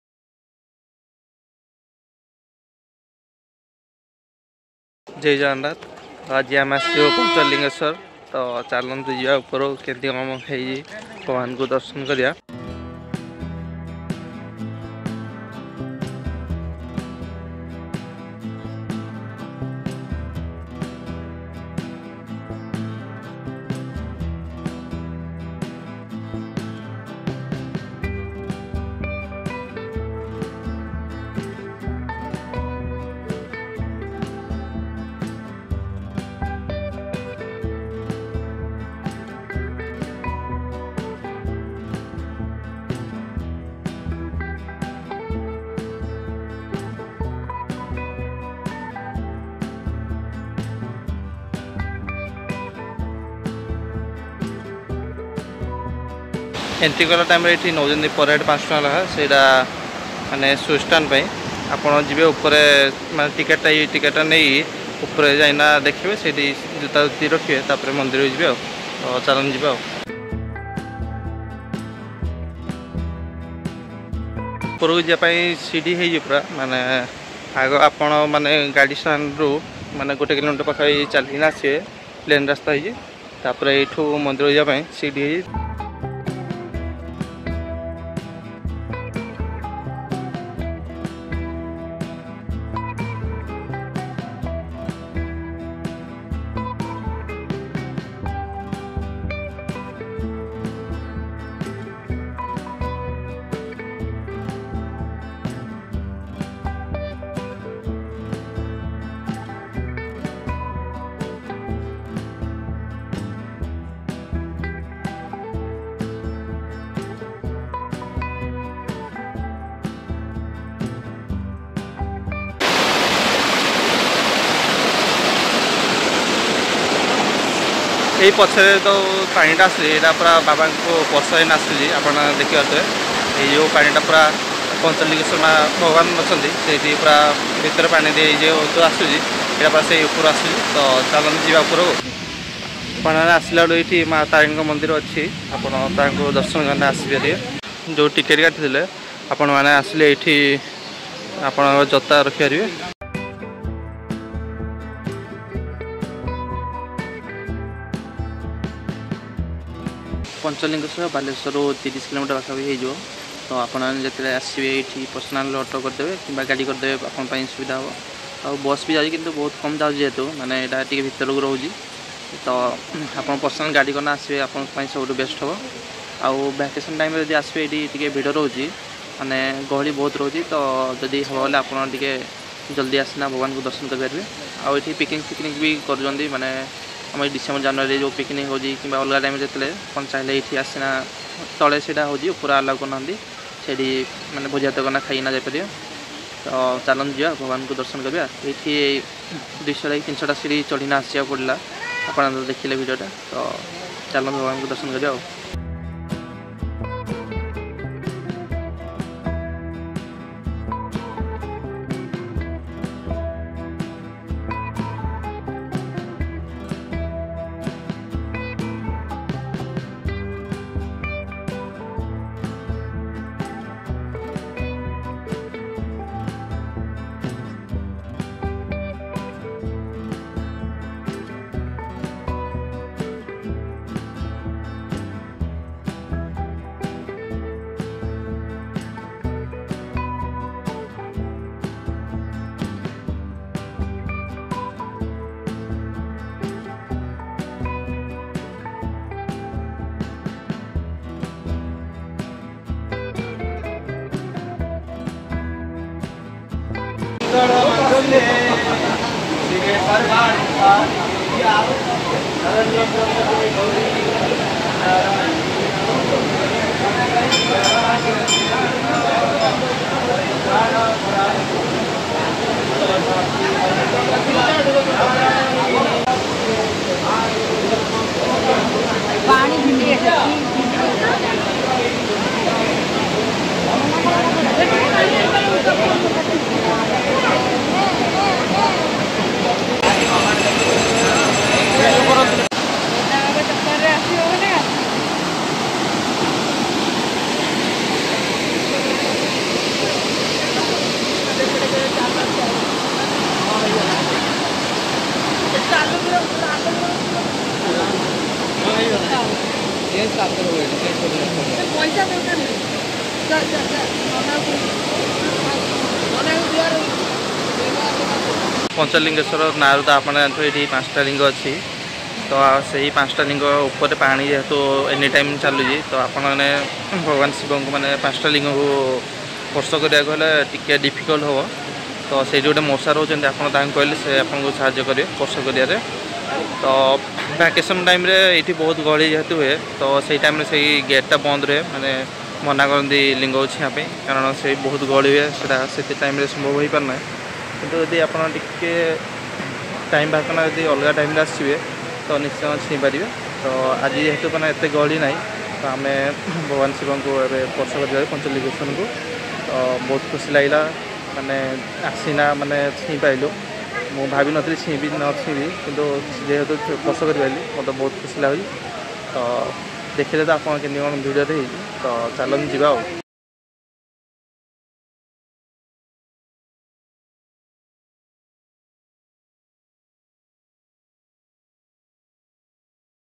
Shree Panchalingeswar Temple Balasore
SHREE PANCHALINGESWAR TEMPLE Balasore Odisha

Best Picnic Spot In Balasore

A Unique Shiv-Linga In ODISHA

ପାଣି ଭିତରେ ଶିବ ଲିଙ୍ଗ
ପଞ୍ଚଲିଙ୍ଗେଶ୍ଵର ଶୈବପୀଠ ବାଲେଶ୍ଵର